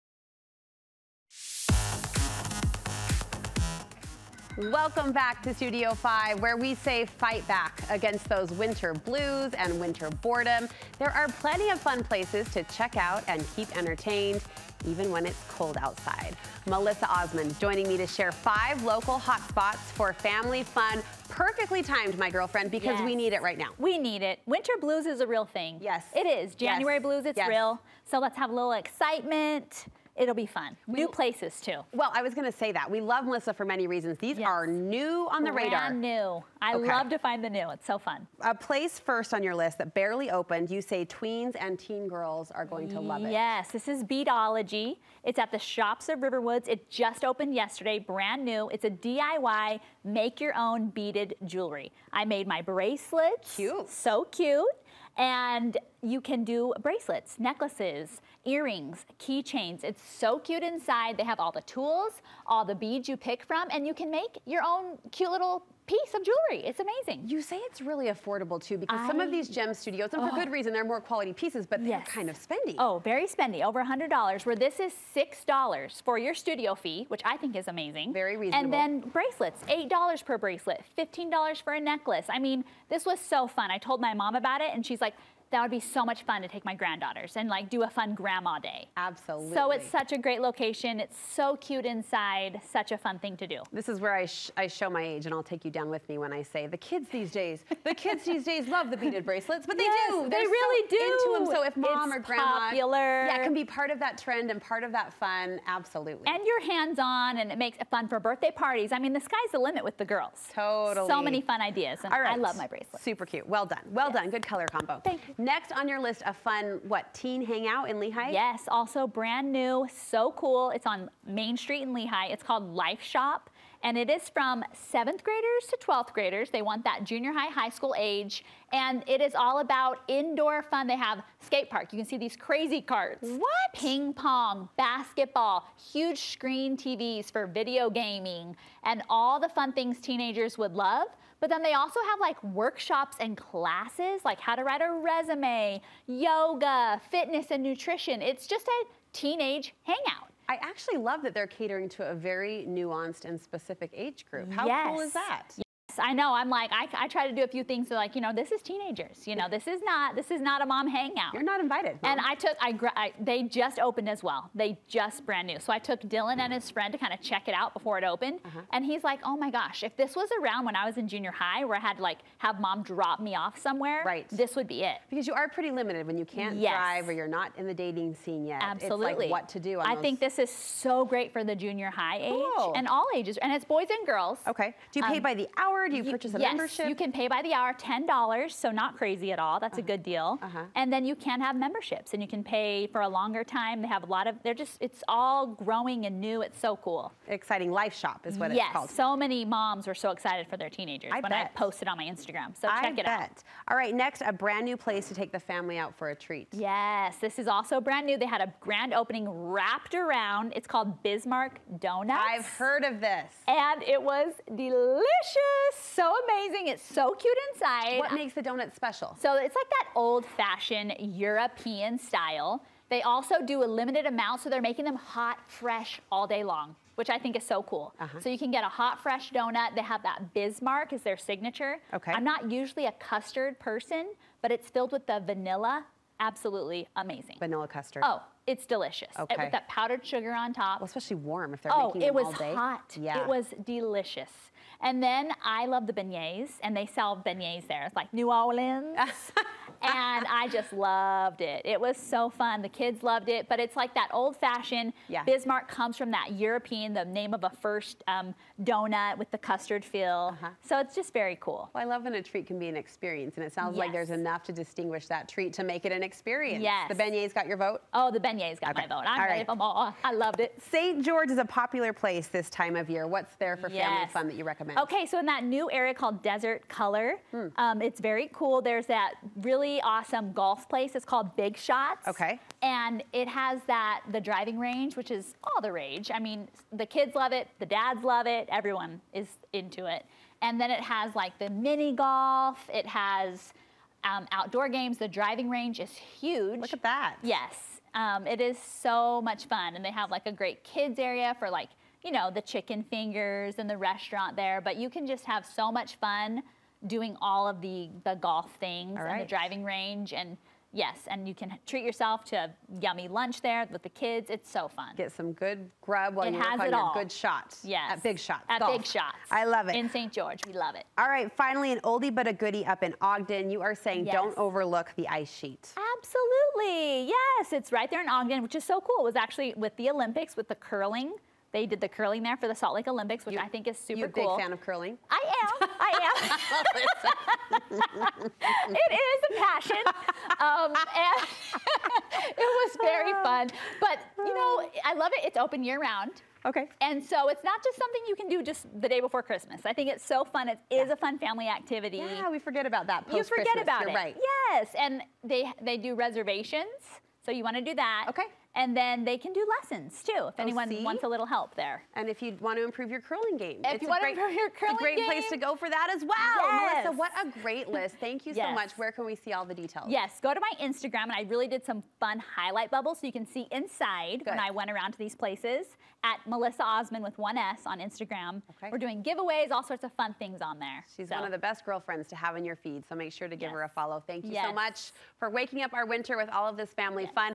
Fight the winter boredom! Try these 5 new activities
Melisa Osmond shares five fun places to check out and keep entertained, even when it's cold outside.

Find more ideas from Melisa on Instagram, @melisaosmond.